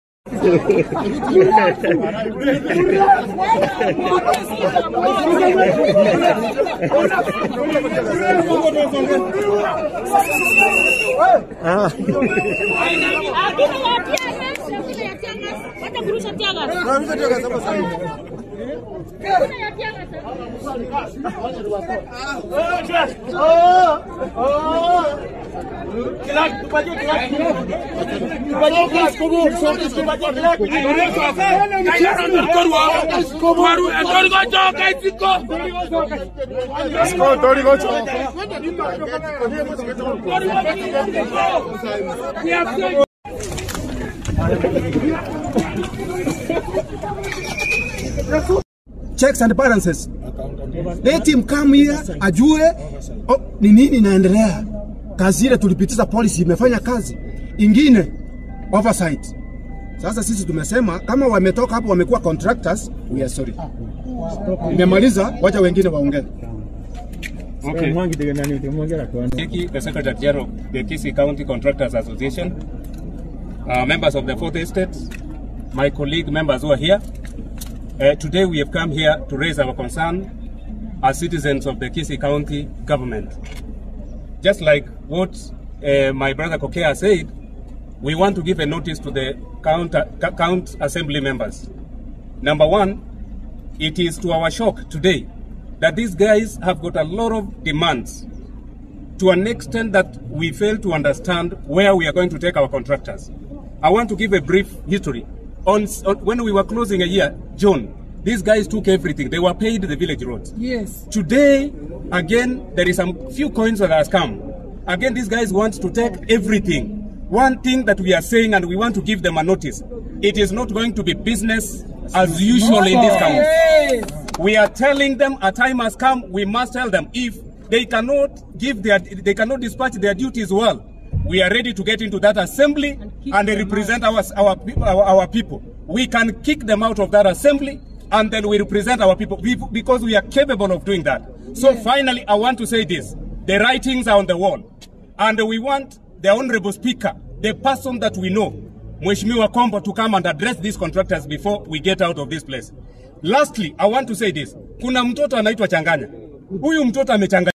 DRAMA AT KISII COUNTY ASSEMBLY AS POLICE OFFICERS LOB TEAR GAS TO DISPERSE CONTRACTORS !!
Police officers in Kisii county have been forced to lob teargas canisters to disperse contractors who had stormed the assembly in protest of irregular awarding of tenders.